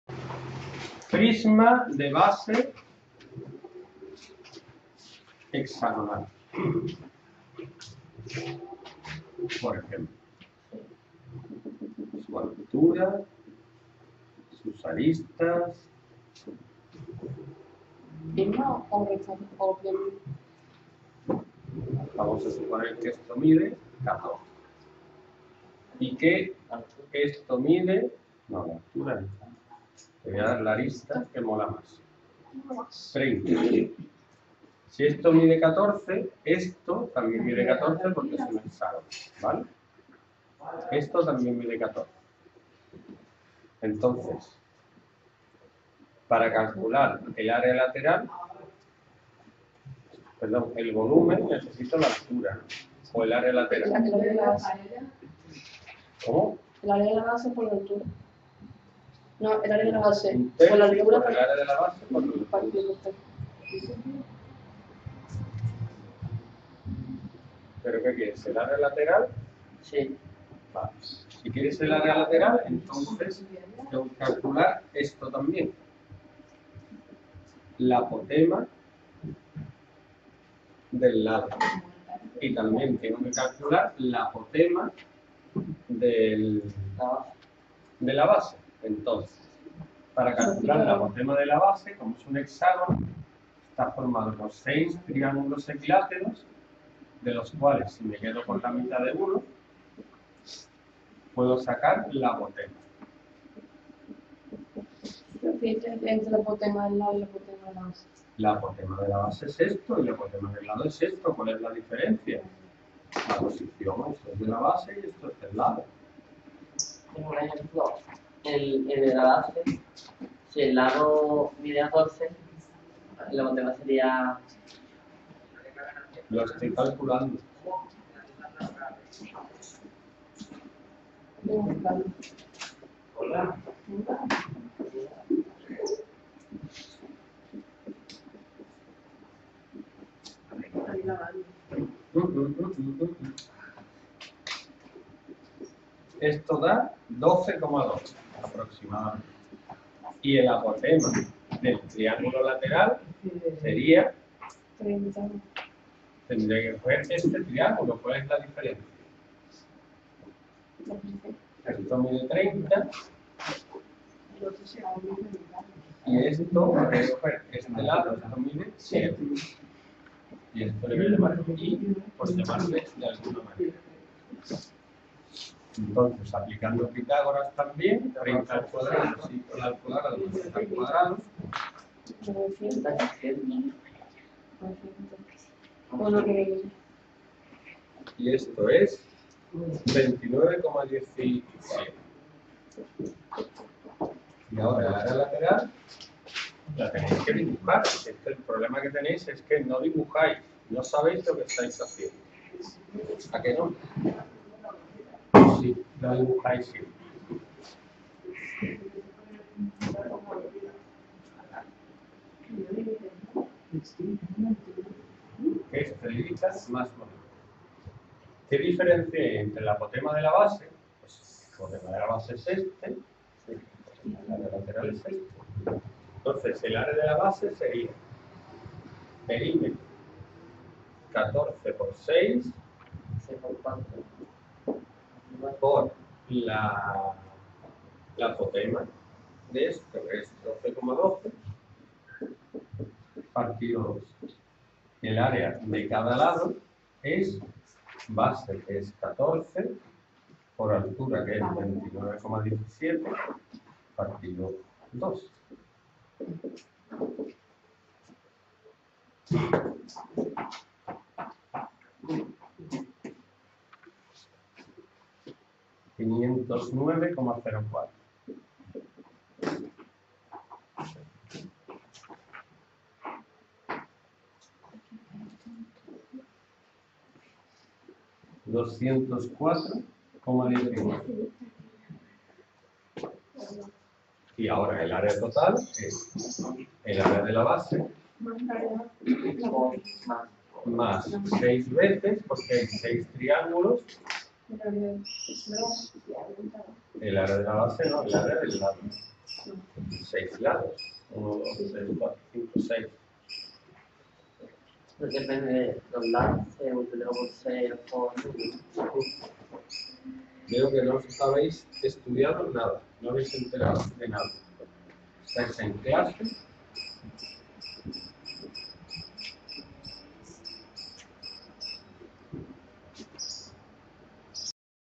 Prisma con base hexagonal Matemáticas 2º ESO Academia Usero Estepona
Prisma con base hexagonal Matemáticas 2º ESO
Academia Usero en Estepona, el centro de formación con más vídeos en Internet del Mundo presenta: Prisma con base hexagonal Matemáticas 2º ESO